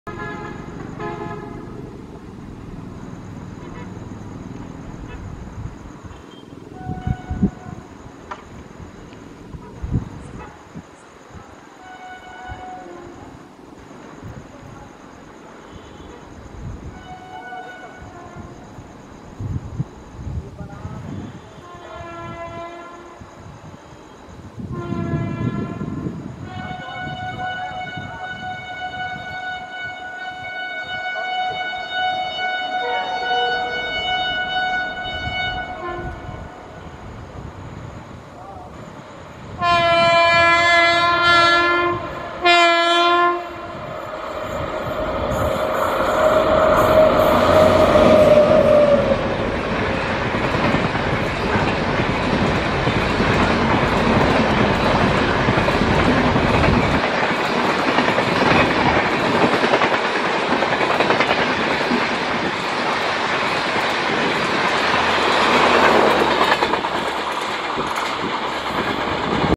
RAJDHANI Rips Past Crawler KURLA!
22691 SBC Rajdhani Express hauled by Lallaguda WAP7#30450 speeds past the looped 11013 Kurla Express hauled by Krishnarajapuram WDP4D#40530 at Rajanukunte!

Device used: Canon SX70 HS.